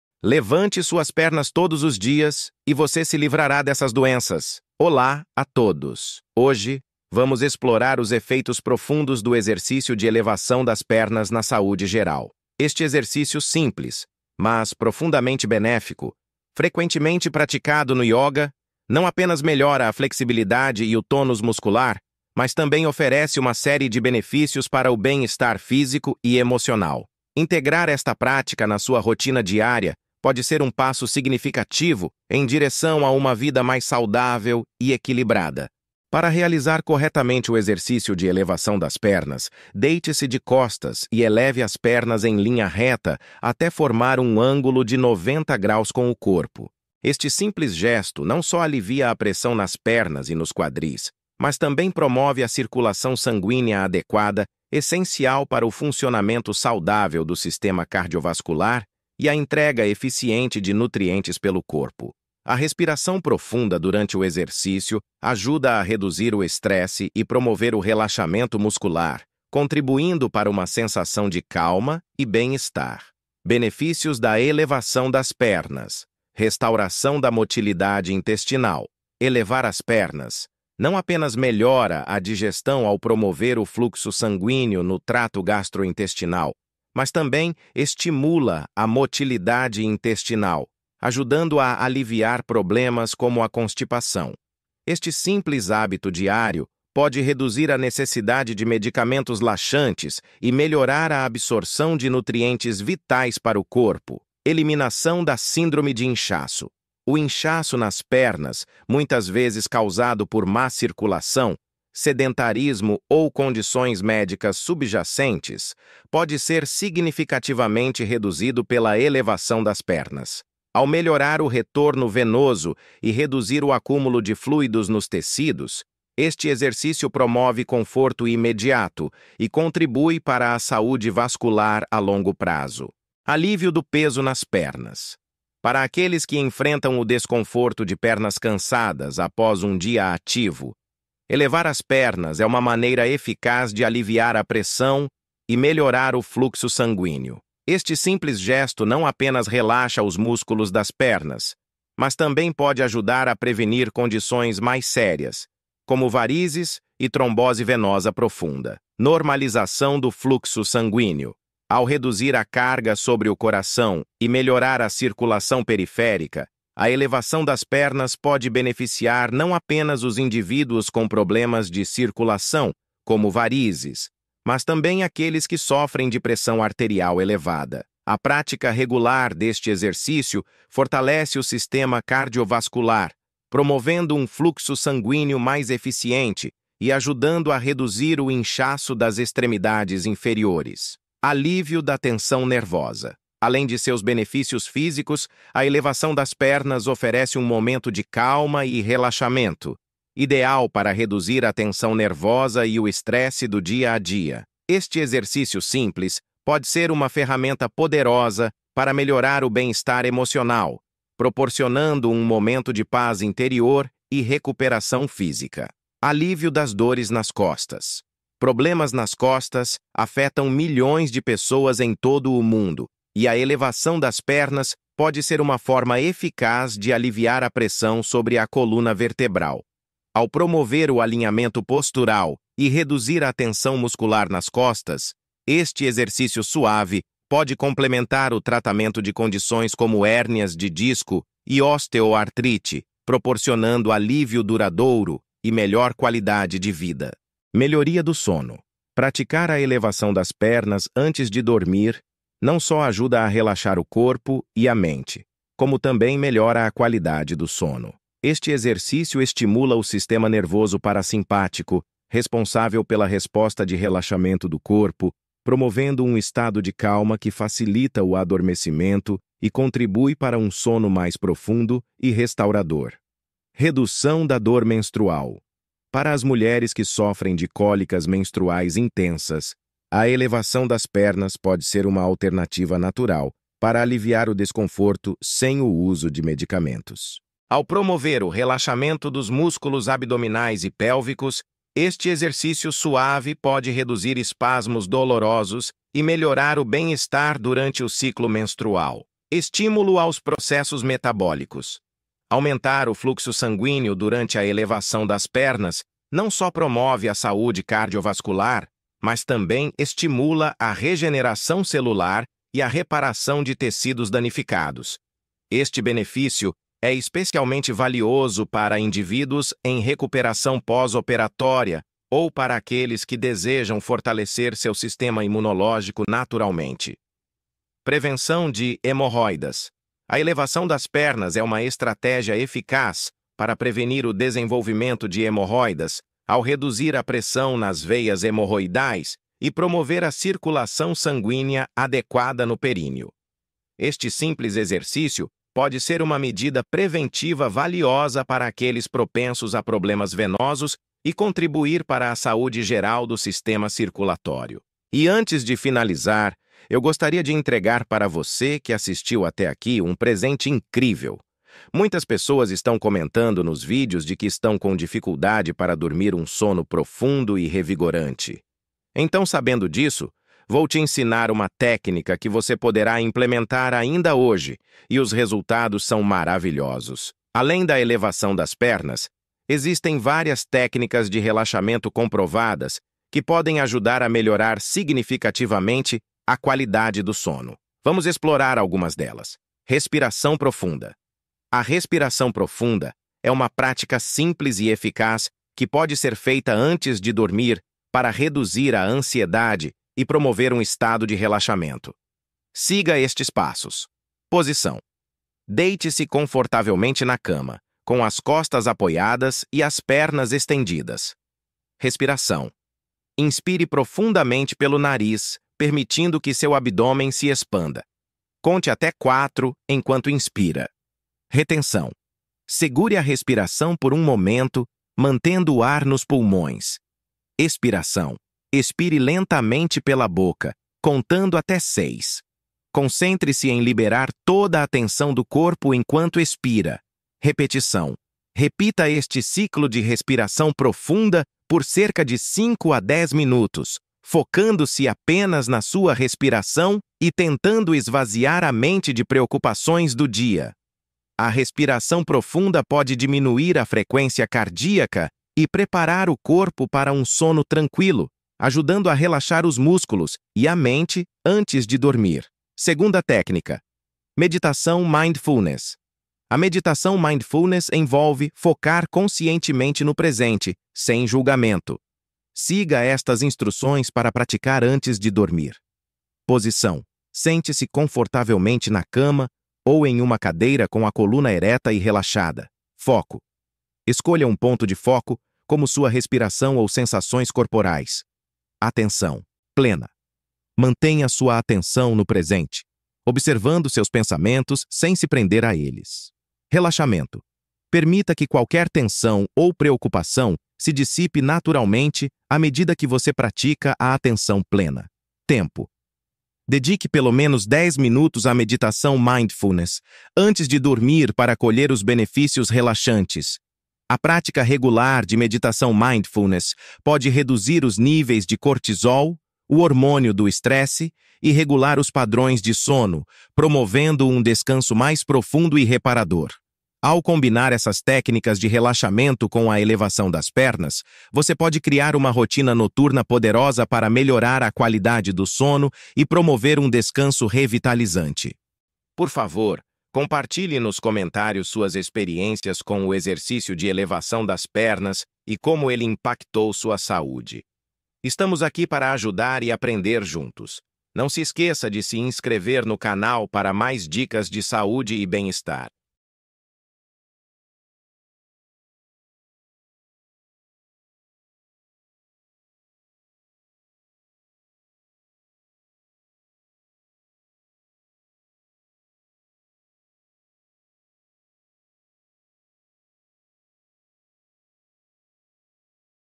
LEVANTE Suas PERNAS Todos Os Dias! E Você Se LIVRARÁ Dessas Doenças
Levante suas Pernas Todos os Dias e Veja os Benefícios! 🌟

🔝 Olá a todos! Hoje vamos descobrir o poder transformador de elevar as pernas diariamente e como esse simples exercício pode livrá-lo de várias doenças. Este hábito, comumente encontrado no yoga, traz uma série de benefícios incríveis para a saúde física e emocional. Integrar a elevação das pernas na sua rotina pode ser o segredo para uma vida mais equilibrada e saudável. Vamos explorar juntos! 🧘‍♂️🌿

📈 Benefícios Incríveis para a Circulação Sanguínea: Deitar-se de costas e levantar as pernas em um ângulo de 90° ajuda a aliviar a pressão nas pernas e quadris, melhorando a circulação. Esse movimento é essencial para um sistema cardiovascular saudável e uma melhor entrega de nutrientes pelo corpo. Além disso, a respiração profunda durante o exercício reduz o estresse e promove relaxamento muscular. 🌬️💪

🌟 Restauração da Motilidade Intestinal e Alívio do Inchaço: Elevar as pernas melhora a digestão e a motilidade intestinal, ajudando a combater a constipação. Esse hábito também pode aliviar o inchaço nas pernas, frequentemente causado por má circulação. Ao melhorar o retorno venoso, reduz o acúmulo de fluidos, promovendo conforto e saúde vascular a longo prazo. 🚶‍♂️🦵

🛏️ Alívio de Dores e Melhora do Sono: Para quem sofre com pernas cansadas, elevar as pernas alivia a pressão e melhora o fluxo sanguíneo, prevenindo problemas como varizes. Este exercício também pode aliviar dores nas costas, promovendo alinhamento postural e relaxamento muscular. E, ao praticá-lo antes de dormir, melhora significativamente a qualidade do sono. 🌙💤

🧘‍♀️ Redução do Estresse e Bem-Estar Emocional: Além dos benefícios físicos, elevar as pernas oferece um momento de calma e relaxamento, ideal para reduzir a tensão nervosa e o estresse diário. Este simples exercício pode ser uma ferramenta poderosa para melhorar o bem-estar emocional, proporcionando paz interior e recuperação física. 😌🌼

🔔 Dicas Extras e Chamado à Ação: No final do vídeo, compartilho uma técnica maravilhosa para melhorar ainda mais seu sono. Não perca! Compartilhe nos comentários suas experiências com a elevação das pernas e como isso impactou sua saúde. Vamos juntos construir uma vida mais saudável! 🎥👍

🌟 Gostou do vídeo? Vamos juntos nessa jornada de saúde e bem-estar. Até a próxima! 🌟💖